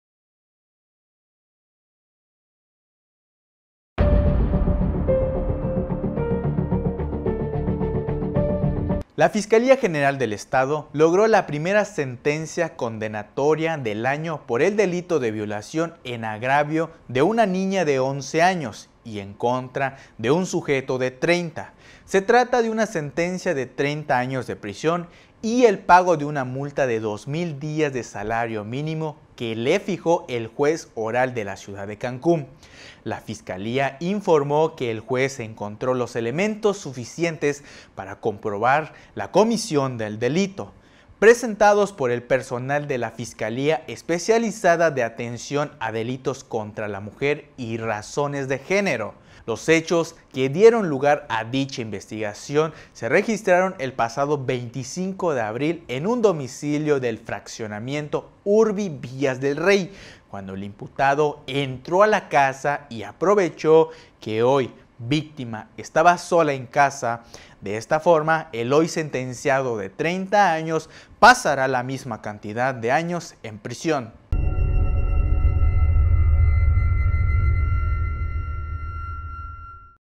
A sus 30 años recibe una condena similar por el delito de violación en agravio de una menor
Entérate de las noticias “EN CORTO” … A sus 30 años recibe una condena similar por el delito de violación en agravio de una menor …